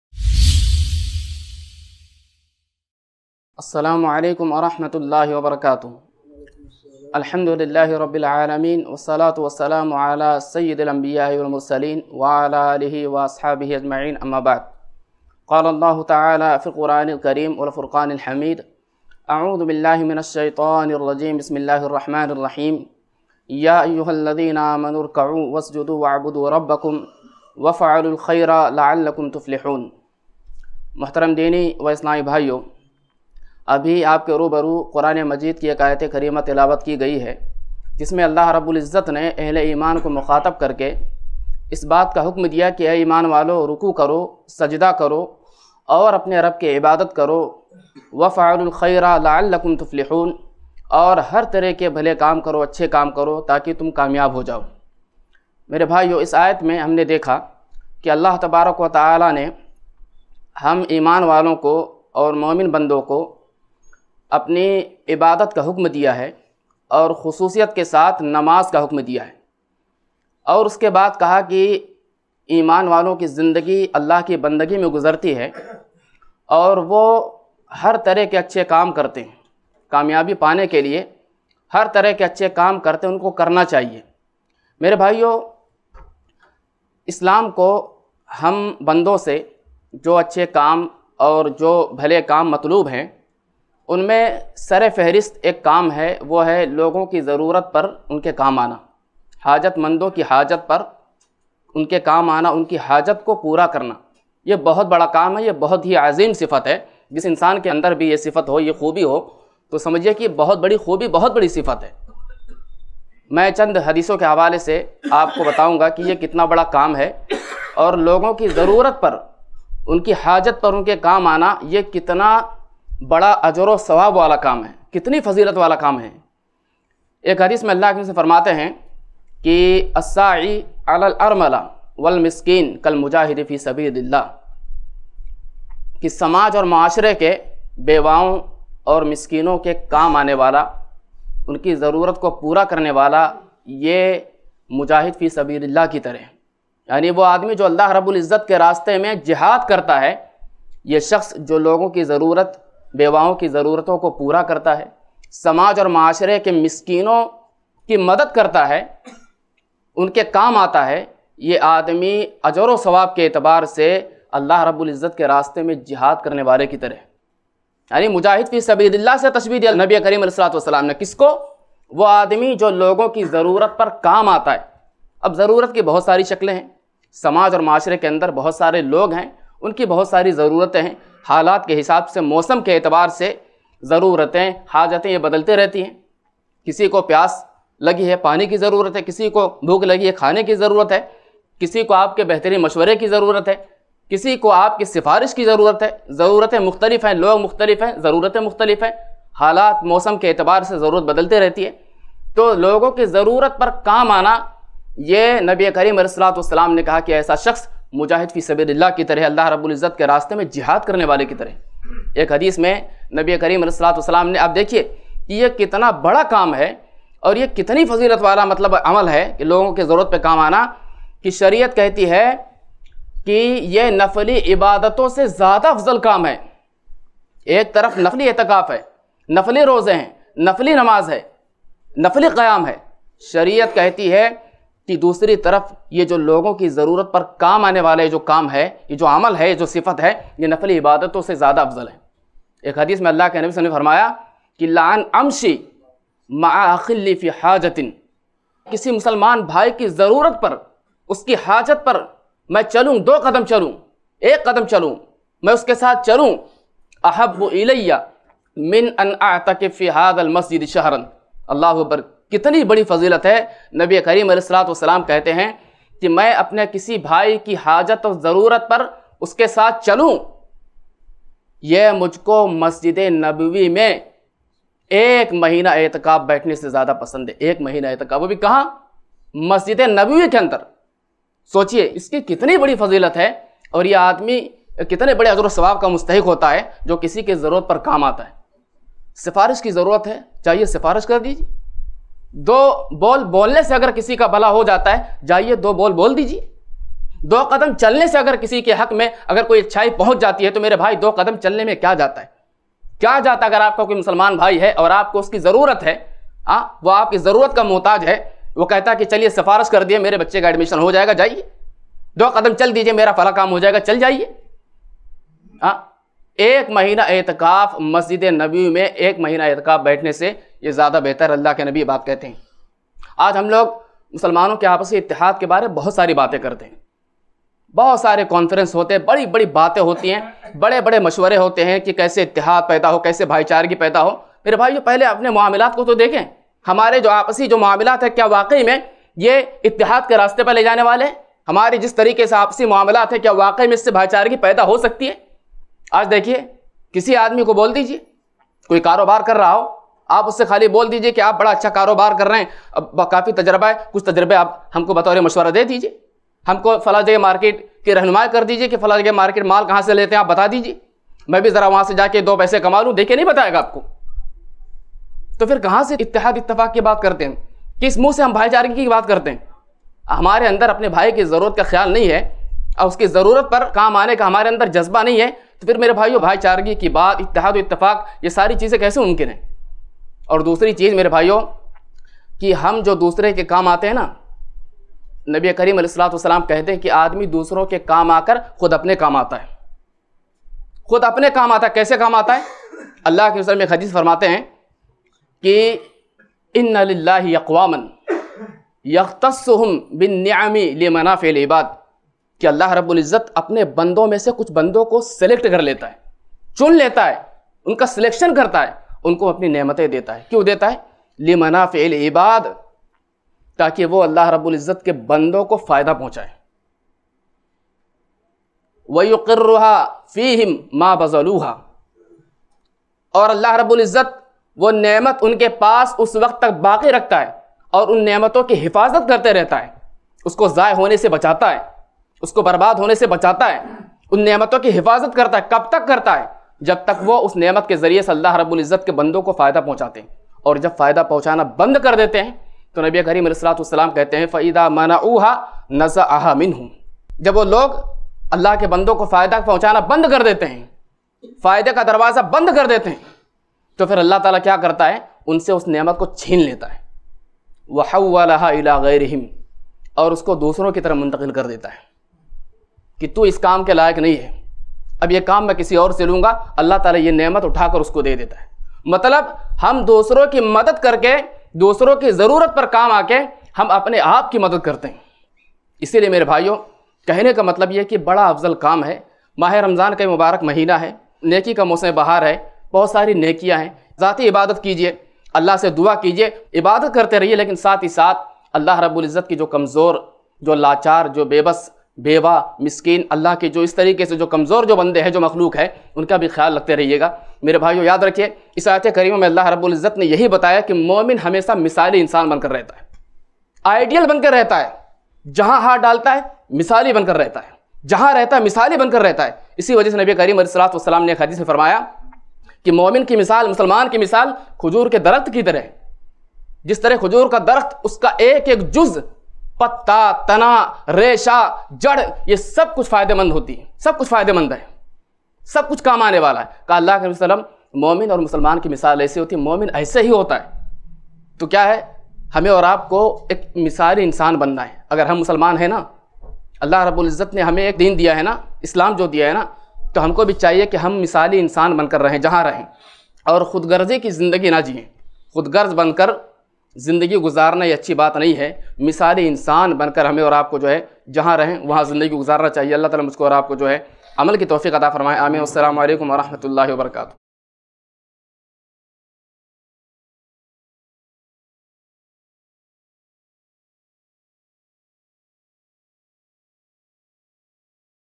Logon Ki Zarurat Par Unke Kaam Aana Itna Bada Kaam Hai | Shaikh Fayyaz Mustaqueem Muhammadi
Bank Account Details:
ILM FOUNDATION
ICICI Bank
Savings account
IFSC CODE-ICIC0001028

Misali Insaan Ban Kar Jiye Logon Ki Zarurat Par Unke Kaam Aana Itna Bada Kaam Hai | Shaikh Fayyaz Mustaqueem Muhammadi | IIC Mumbai